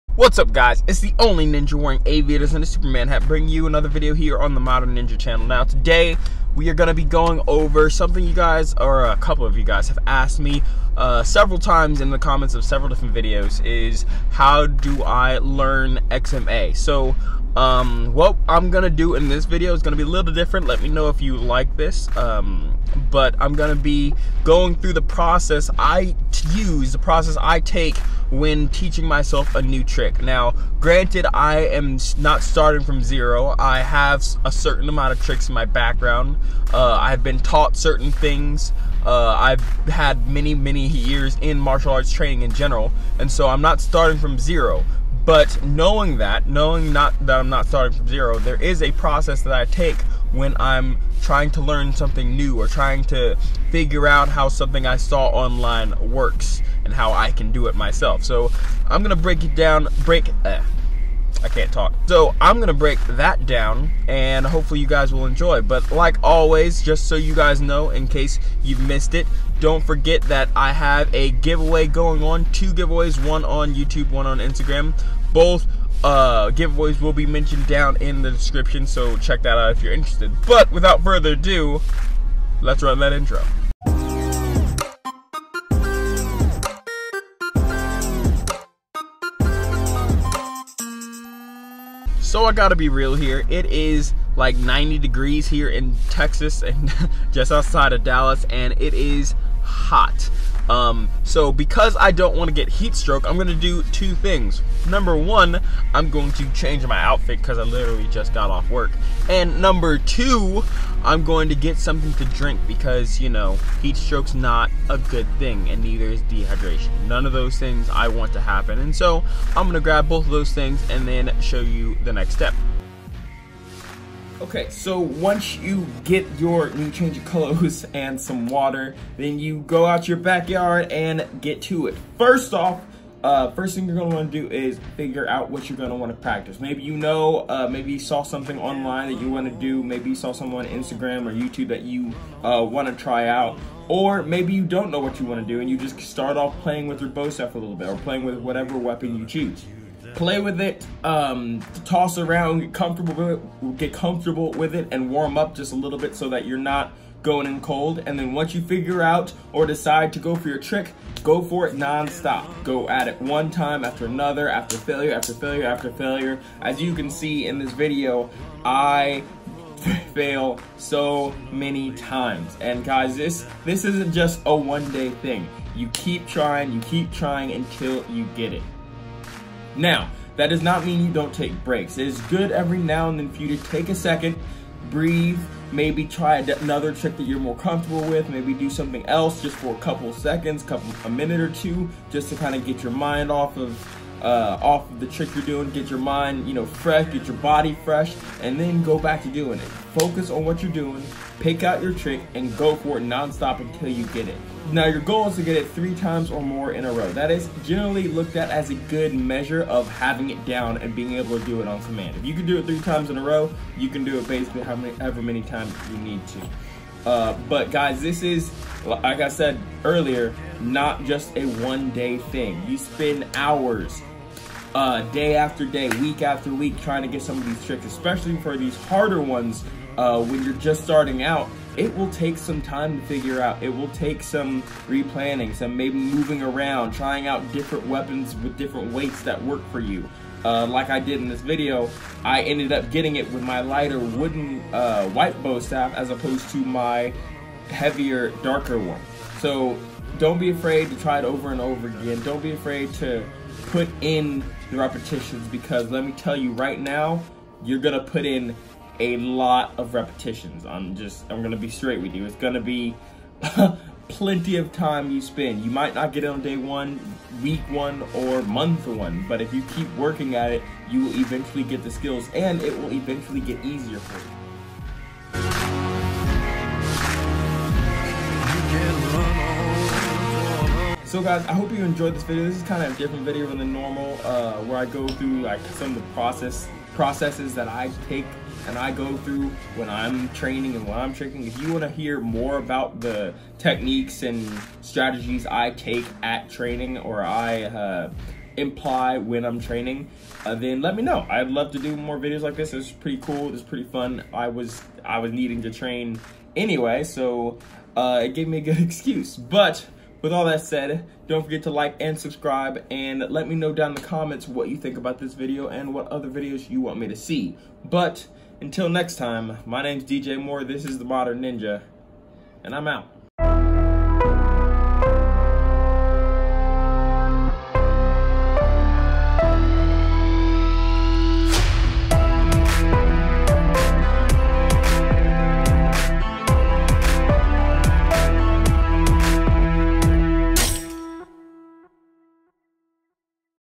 My XMA Training Process
So a bunch of you guys have asked me how you can teach yourself XMA and weapons tricks. The best option is to find a teacher, but for those of you that cant, here is one of the processes I use to teach myself new skills.